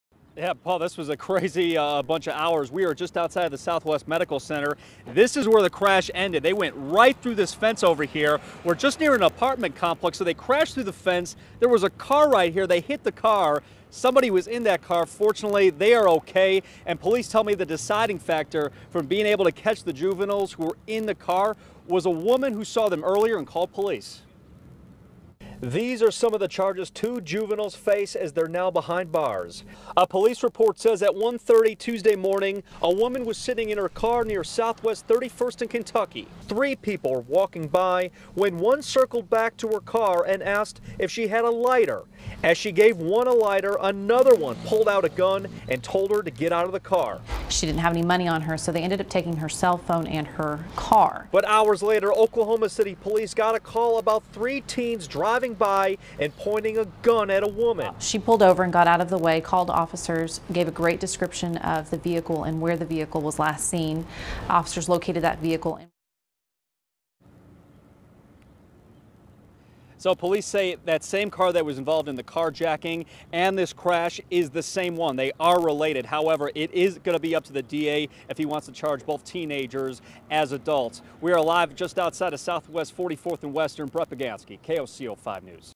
Teens steal woman’s car at gunpoint, arrested after pursuit ends in crash, police say
Two teenagers have been arrested after an apparent crime spree in Oklahoma City that police say started with an alleged carjacking early Tuesday morning.